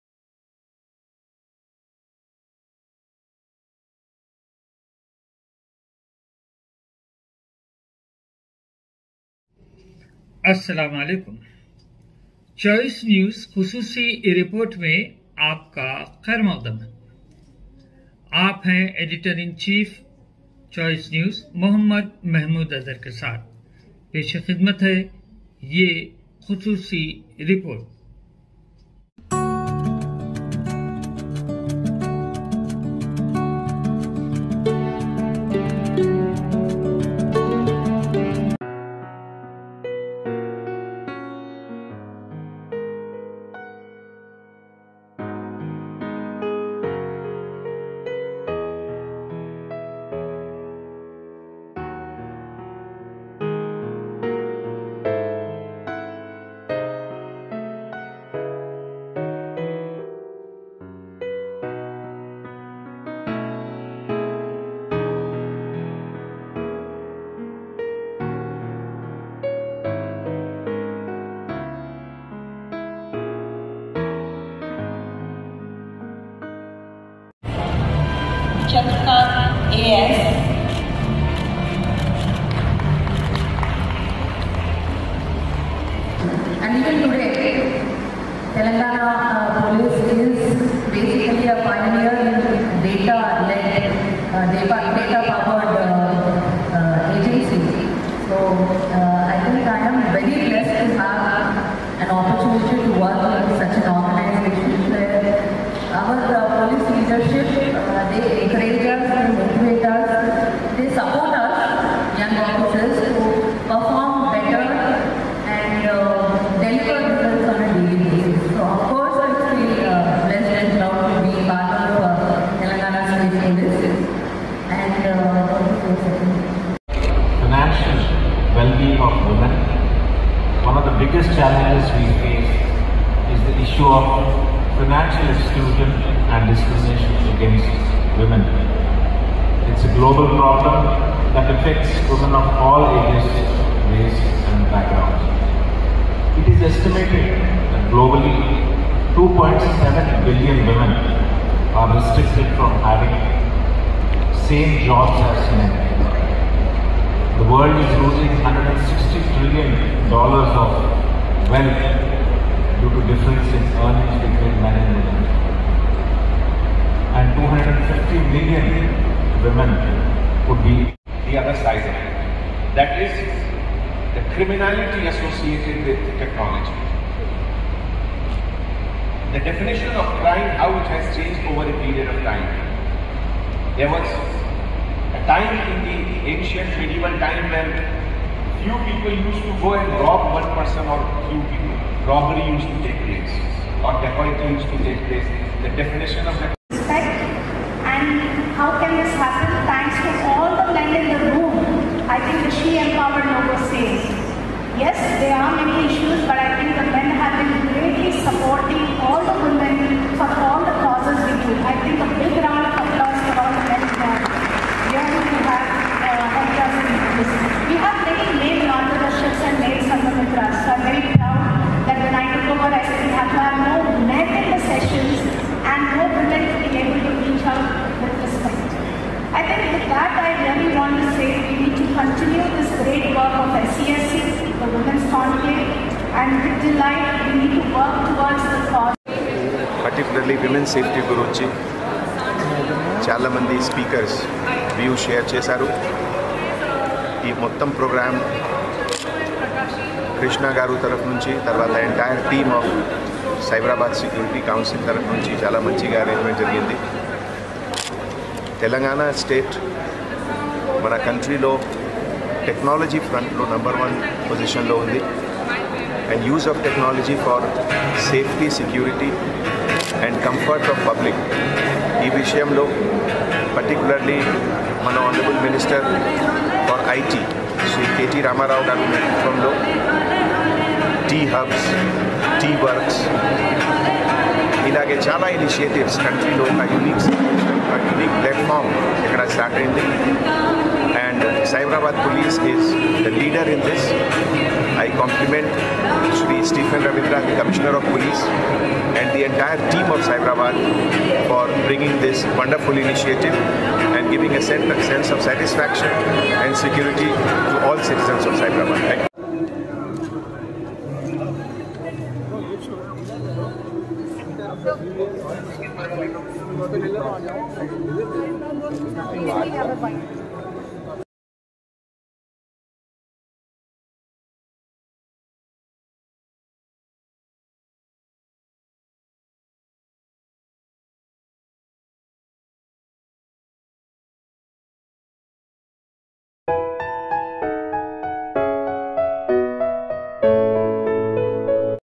5th Women's Conclave & Awards 2023.Held at JRC Convention Center Hyderabad Telangana.
5th Women's Conclave & Awards 2023.Held at JRC  Convention Center Hyderabad Telangana.(Report:MD.Azher Uddin.Choice News)